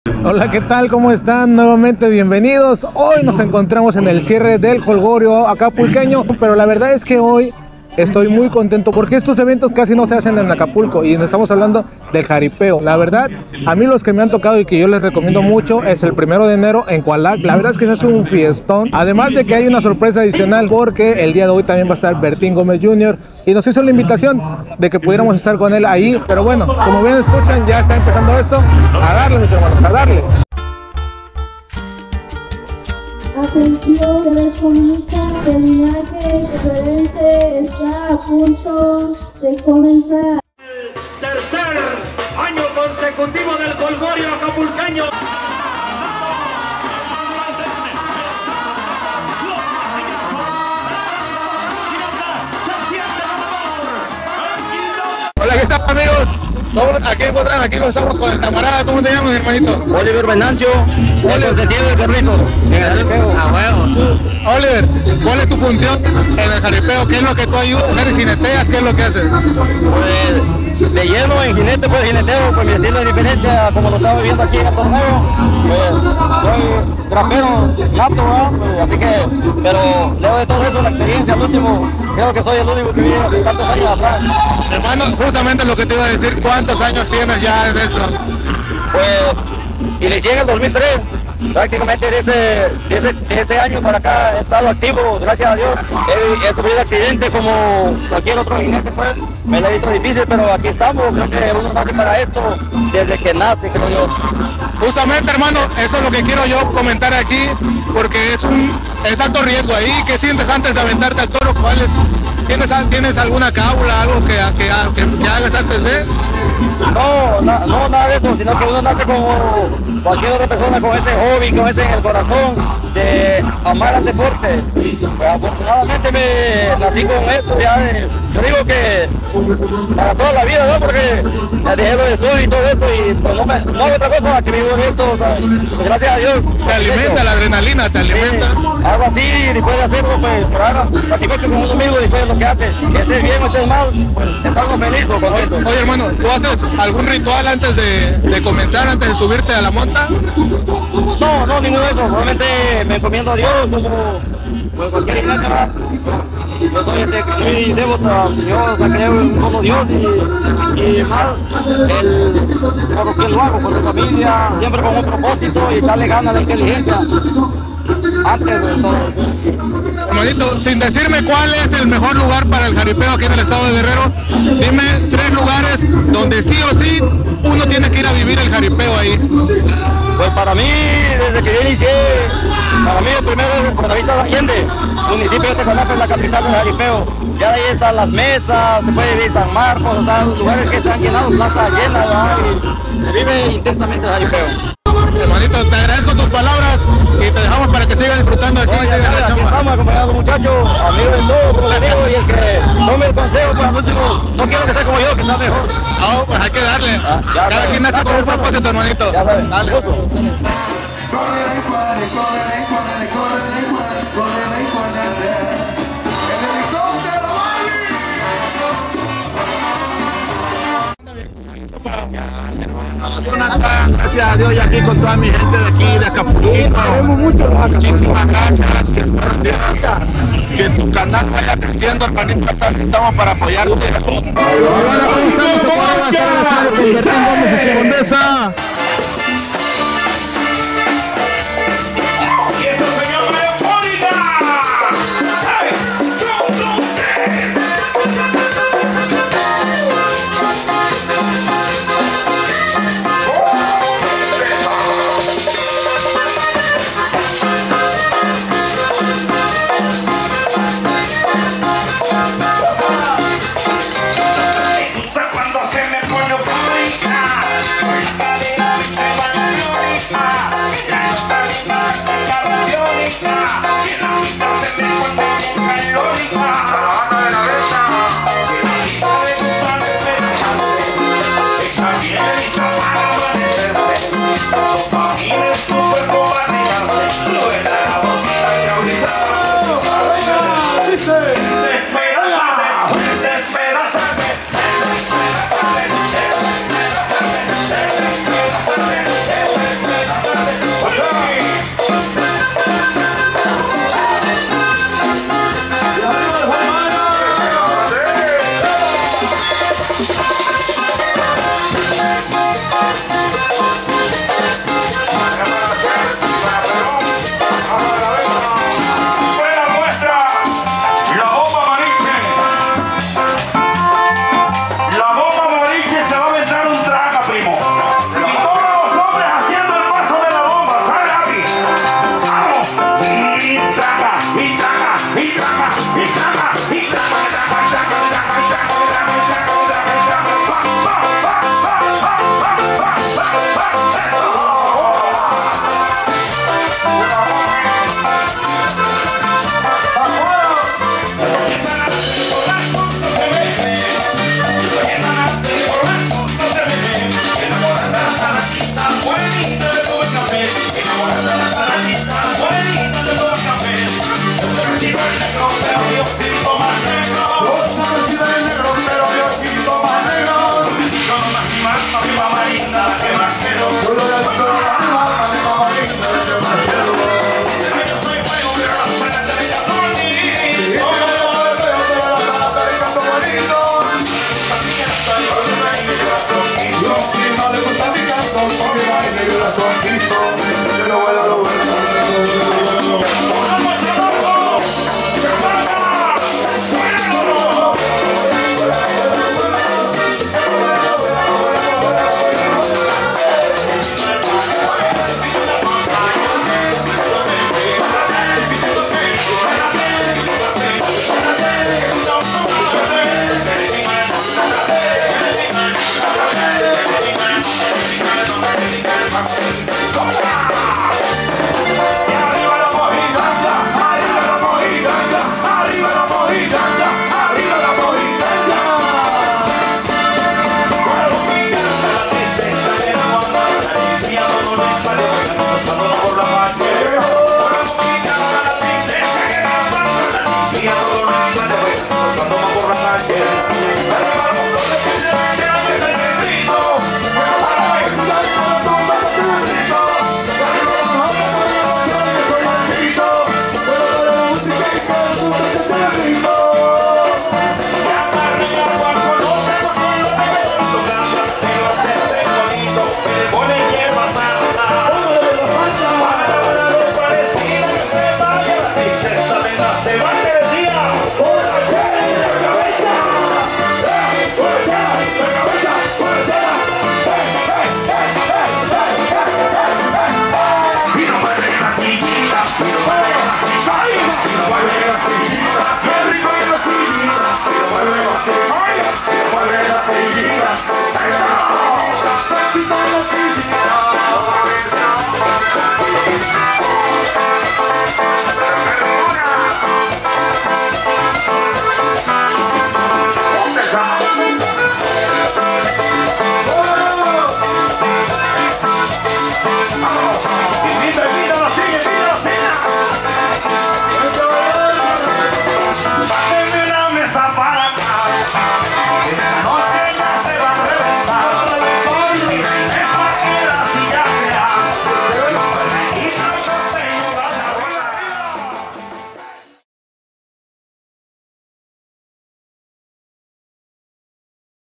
Bertin Gomez Jr nos invito a grabar al escenario, Pura buena vibra con el Condesa
Mis hermanos nos fuimos con Bertín y su condesa al cierre del jolgorio acapulqueño 2024 ¿Quieren saber como nos fue? acompáñame en el video.